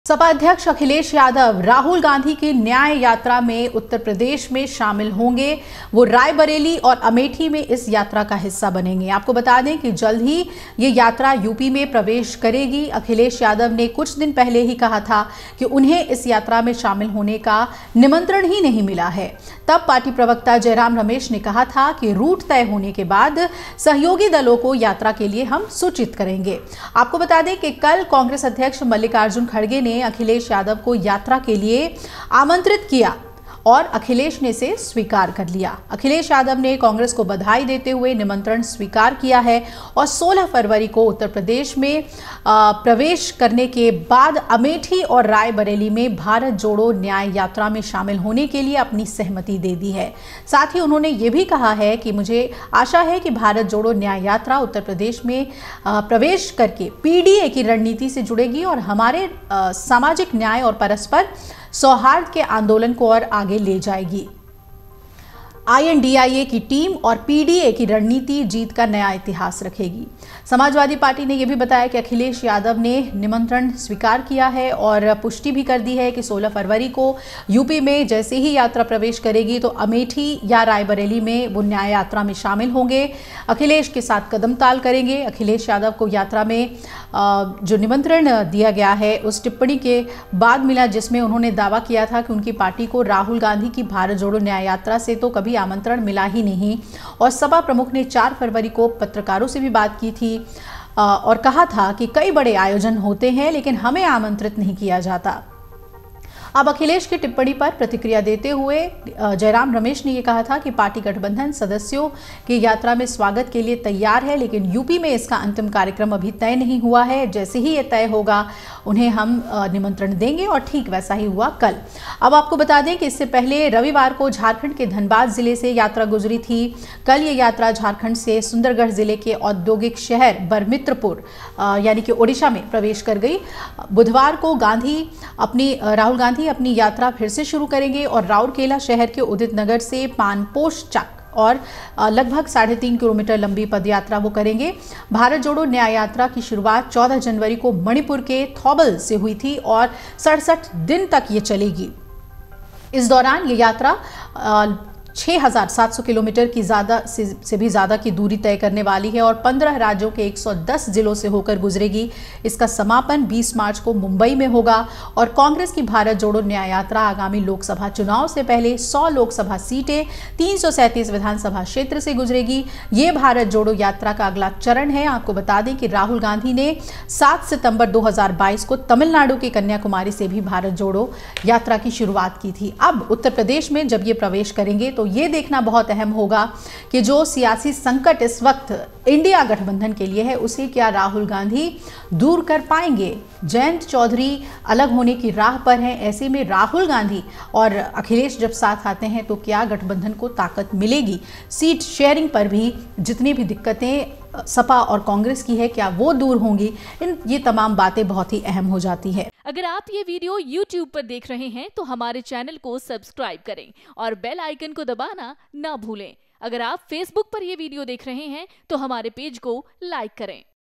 Bharat Jodo Nyay Yatra: UP में Rahul Gandhi के साथ Akhilesh Yadav यात्रा में शामिल होंगे।
सपा अध्यक्ष अखिलेश यादव राहुल गांधी की न्याय यात्रा में शामिल होंगे। वह रायबरेली और अमेठी में इस यात्रा का हिस्सा बनेंगे। जल्द ही यह यात्रा यूपी में प्रवेश करने वाली है।